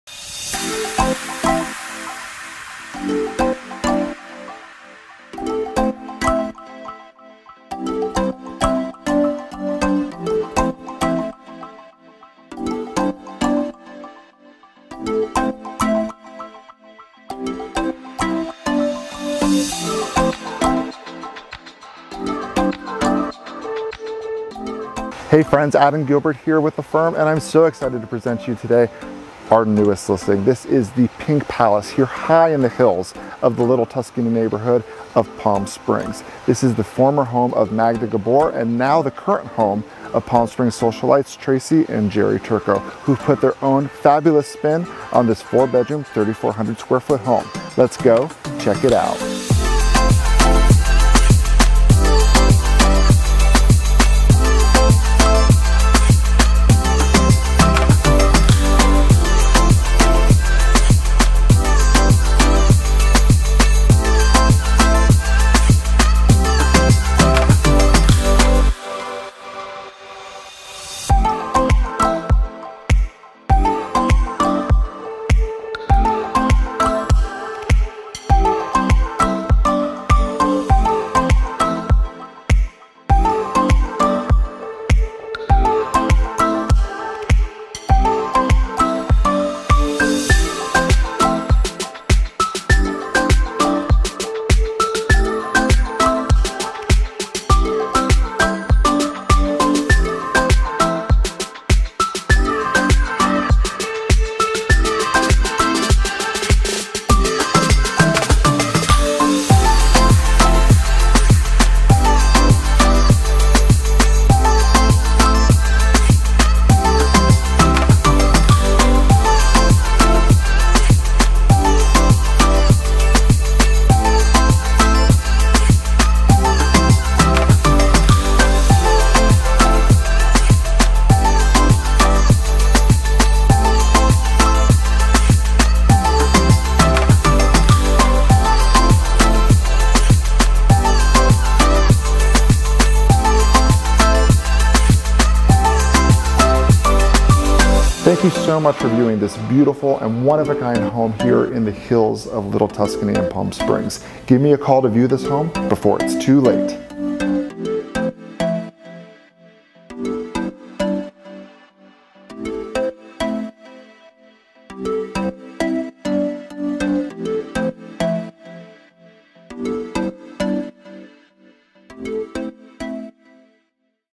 Welcome to "The Pink Palace" - Former home to the Gabor Sisters!
Introducing "The Pink Palace," former home to Old Hollywood actresses Eva, Magda, and Zsa Zsa Gabor. Purchased in 1969, the Gabor residence quickly became an iconic landmark as the girls made a strong presence throughout the greater Palm Springs area. Their taste for the finer things in life, showed quite clearly in their ritzy and elegant interior design style. Later remodeled and brought back to life by owner and international designer Tracy Turco in 2021, this unique property perfectly encapsulates the blend between Old Hollywood glam and mid-century modern style architecture.

Perched on more than a half-acre lot atop Little Tuscany Estates, this spacious property has four bedrooms, three bathrooms, and an open floor plan with a signature Turco Twist. Inside, you'll find various original furnishings and art pieces from the Gabor sisters themselves, such as a baby grand piano and several oil paintings from world renowned artists. The dark colored kitchen with marble countertops is the perfect contrast to the overall vibrant pink exterior. Cheetah print walls and luxury inspired bedrooms offer an inviting and nostalgic experience that makes you feel as though you're living the Hollywood life.

Unwind in the Master bedroom with a custom made round plush bed, black and gold feathered accent walls, spacious walk-in closet, and floor-to-ceiling windows from the bathroom that allow you to oversee all of Palm Springs. Outside, you're greeted with unparalleled 360 mountain views and a taste of elegance with Italian-inspired marble statues near the pebble-tech pool. Relax by the fire pit in the outdoor lounge area, or visit with guests and enjoy the desert scenery. Featured in this year's Modernism Week, this historic piece of property holds a very special value, and is meant for those who can appreciate the boundless limits of art and design. Reach out today for a private showing of one of the most sought after properties the desert has to offer!